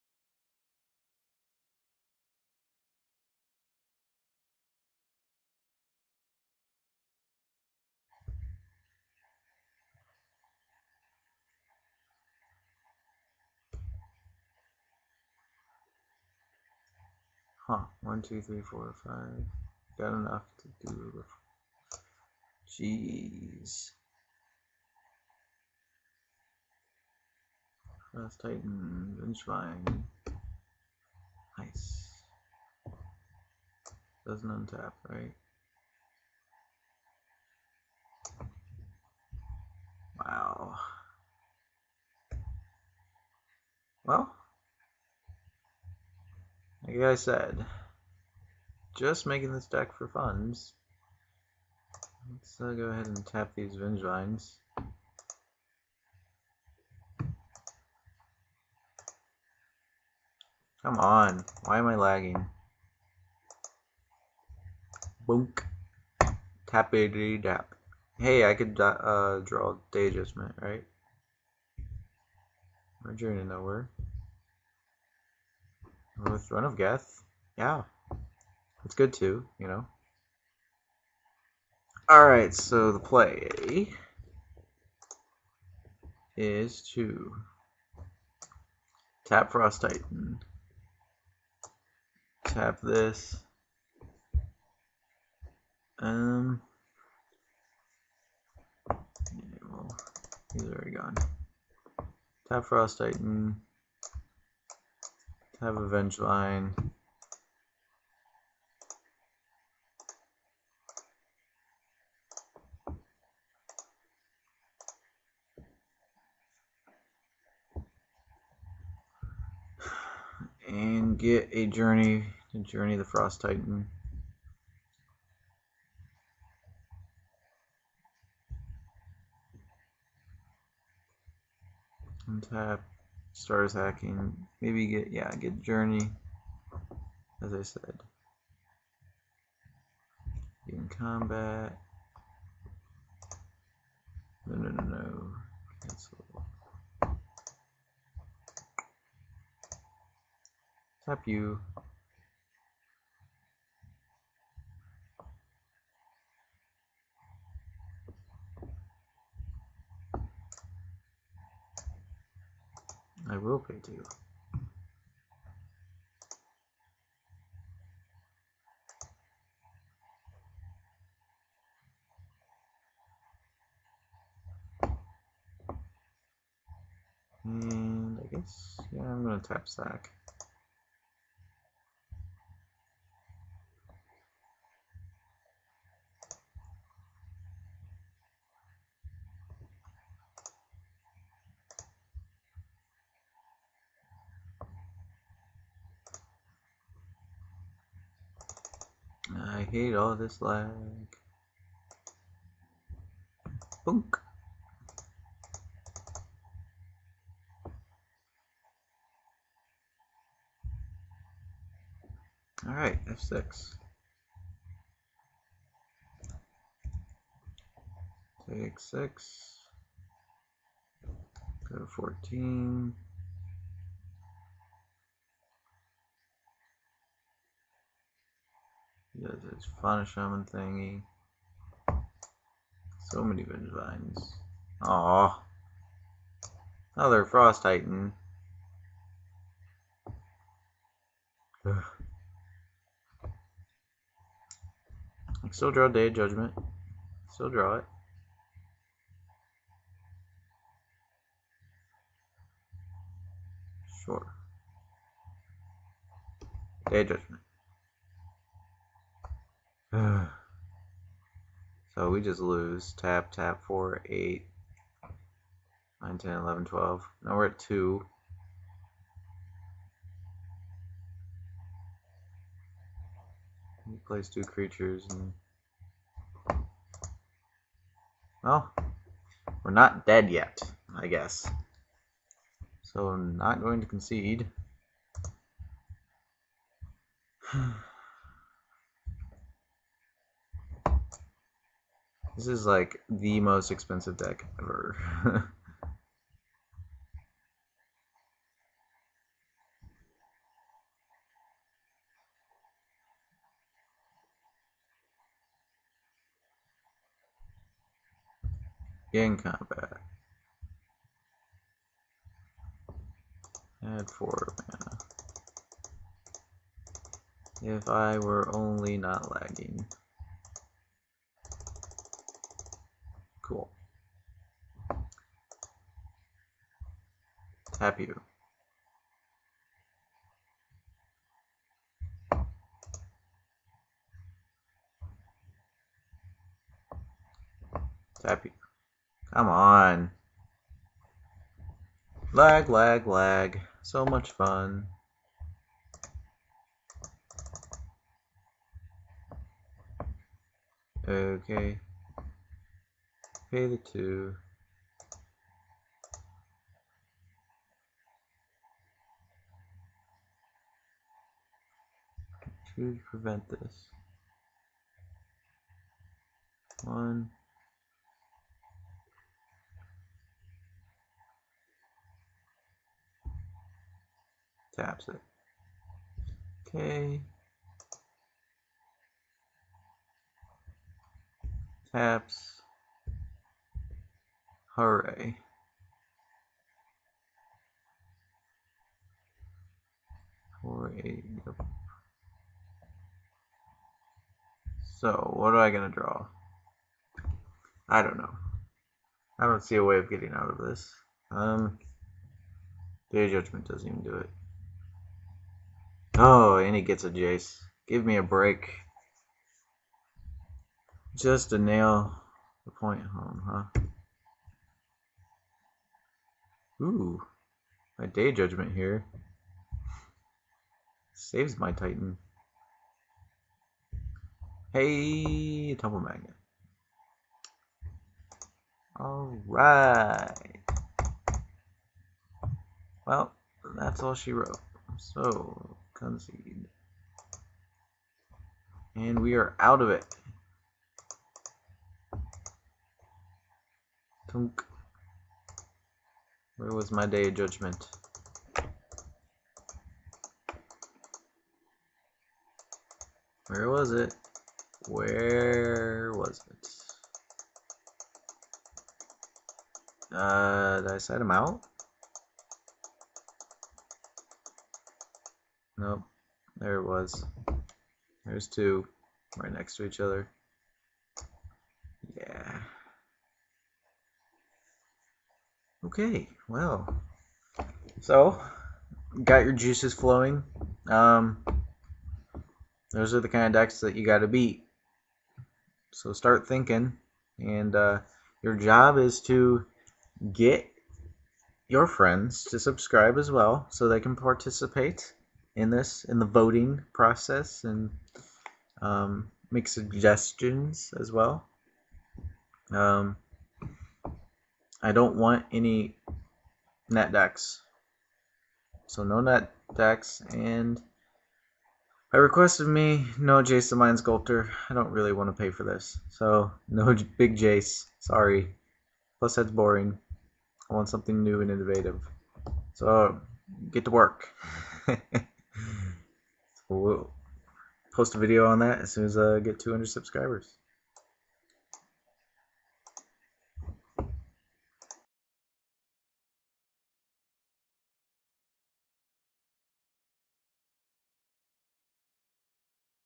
[TCM]: Constructed - ProLum (Part 5 of 5)
This is a deck I created in one day. This is only meant to inspire you to start deckbuilding as it is not a "net deck." It is not meant to be very legitimate but more fun. See the intro of the first video in this series for more info.